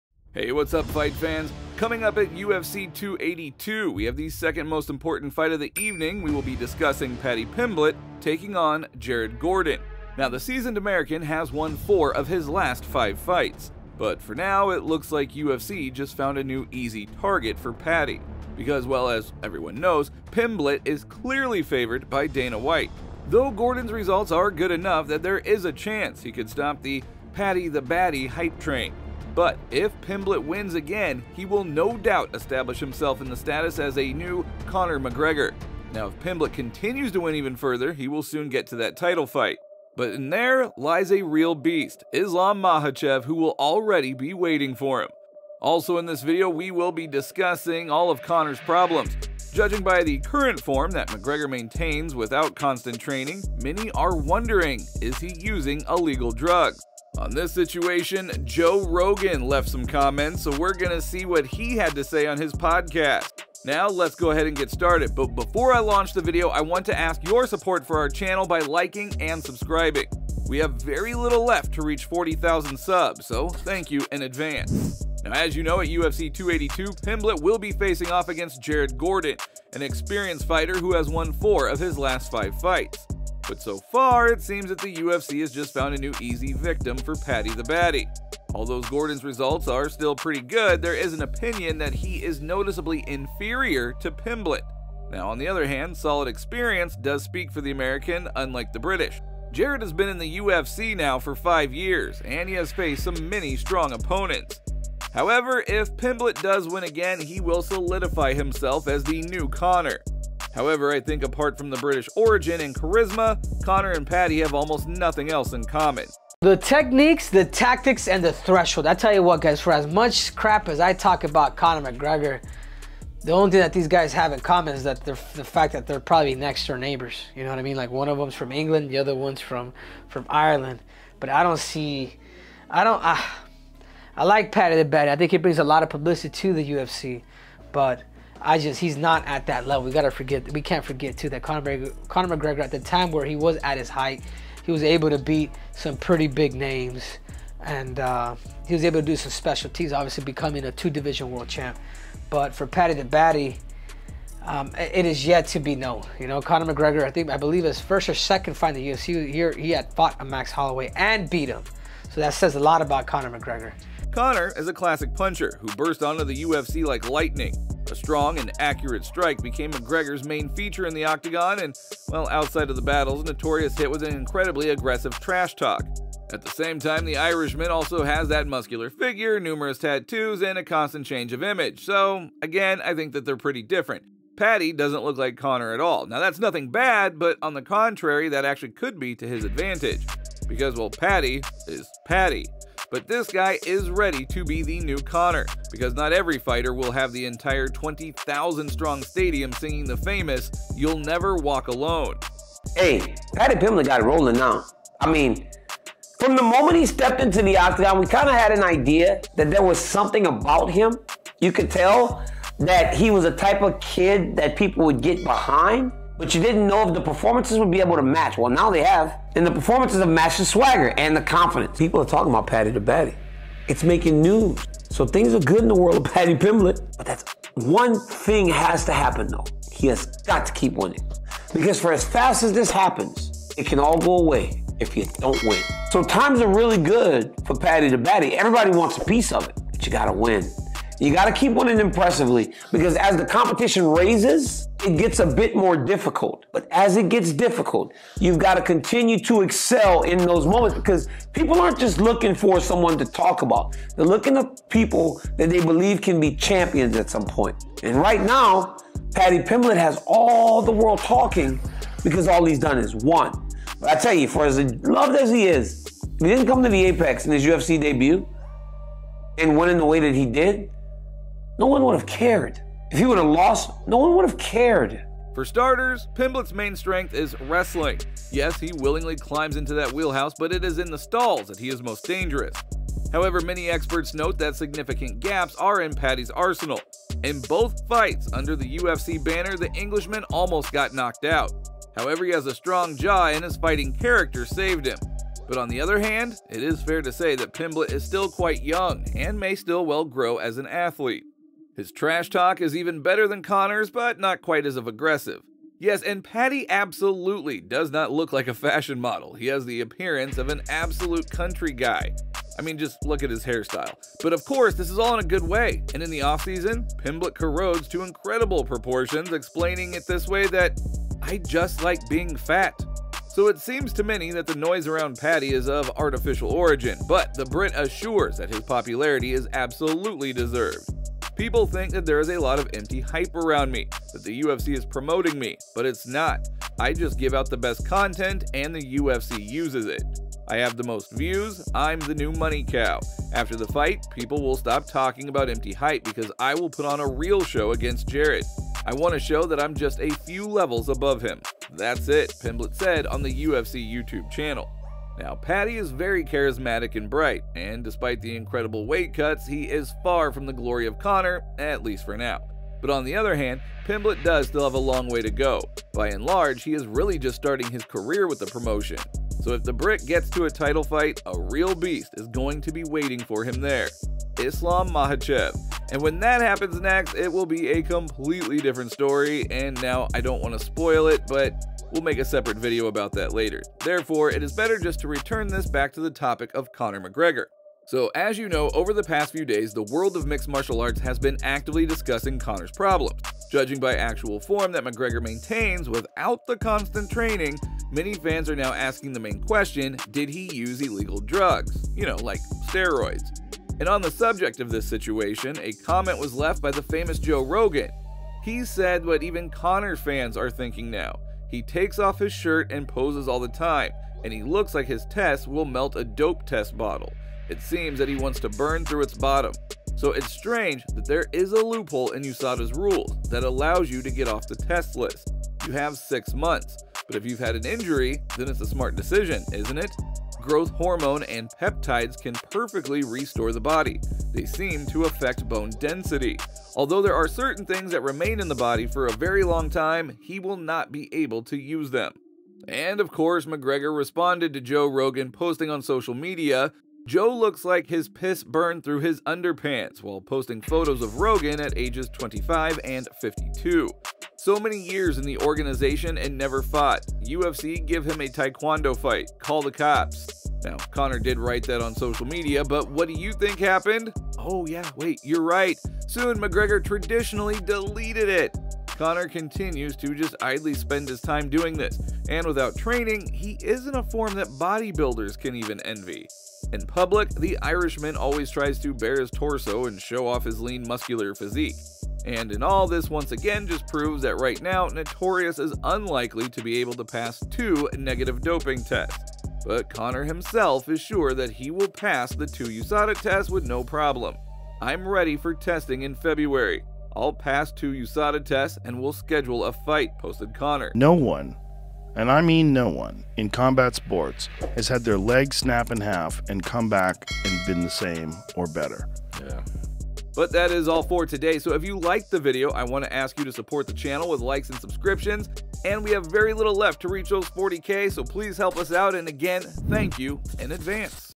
BIG NEWS: ''Conor McGregor Shocked Fans'' Paddy Pimblett Plays Conor McGregor.
Today in this video. Upcoming UFC 282, in the second most important fight of the evening, Paddy Pimblett will take on Jared Gordon, the seasoned American who has won four of his last five fights. But for now, it looks like UFC just found a new easy target for Paddy. Because, as everyone knows, Pimblett is clearly favored by Dana White. though Gordon's results are good enough that there's a chance he could stop the Paddy Pimblett's hype train. But if Pimblet wins again, he will no doubt establish himself in the status of the “new Conor Maggregor”. But, if Pimblett continues to win further, he will soon get to the title fight. But there, a real beast, Islam Makhachev, will already be waiting for him. Also in this video. Over the past few days, the world of mixed martial arts has been actively discussing Conor's problems. Judging by the current form that McGregor maintains without constant training, and many are wondering. Did he still use illegal drugs? On this situation, the famous Joe Rogan left comments, who did not pick up expressions, and he said in his potcast what even Conor fans are thinking now: So, let's start in order. So thanks in advance.